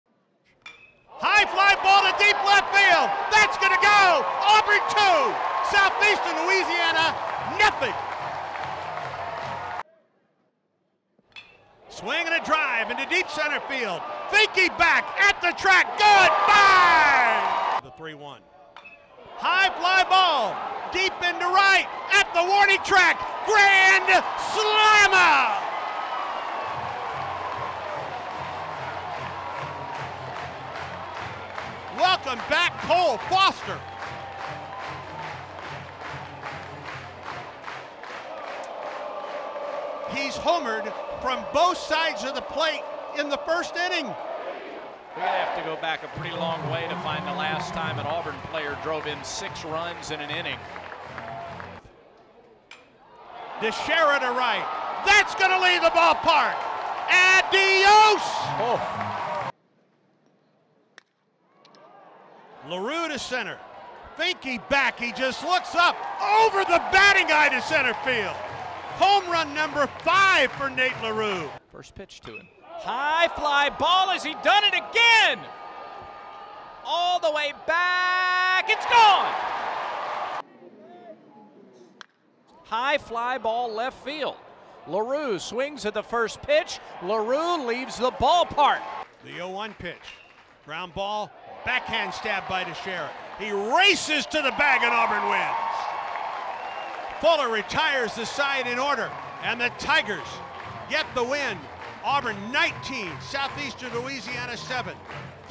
Baseball: Auburn 19, Southeastern Louisiana 7
Cole Foster crushed three of Auburn's seven home runs and drove in nine to lead the Tigers to a 19-7 victory over Southeastern Louisiana on Friday in the opening day of the Auburn Regional at standing-room-only Plainsman Park.